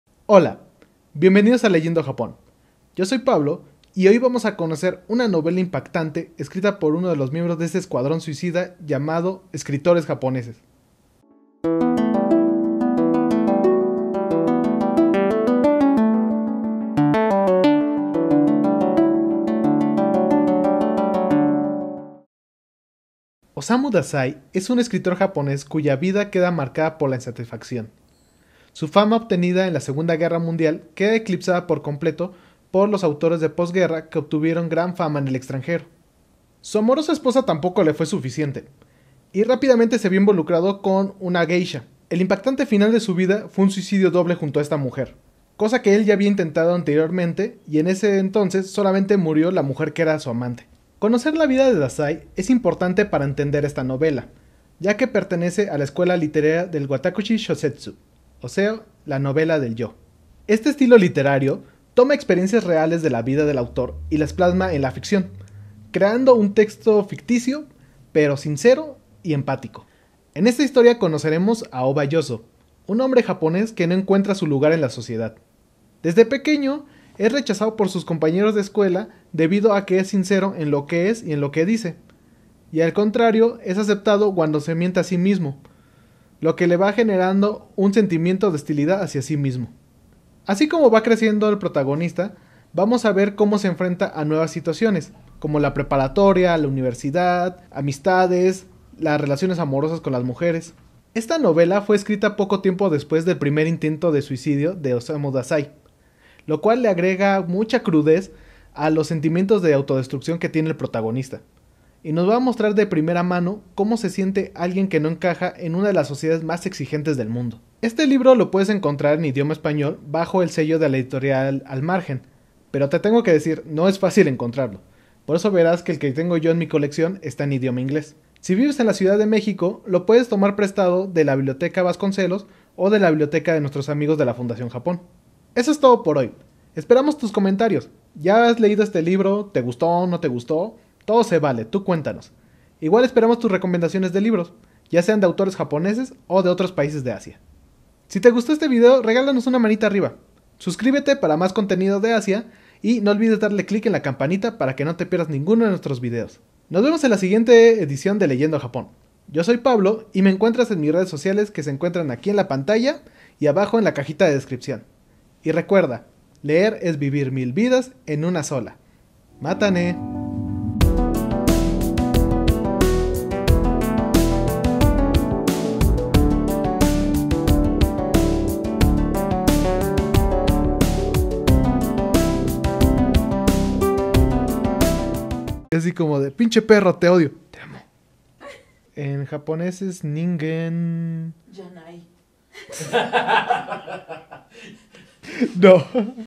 Reseña "Indigno de ser humano" de Osamu Dazai
En esta ocasión reseñamos la obra de uno de los miembros del escuadrón suicida de escritores japoneses.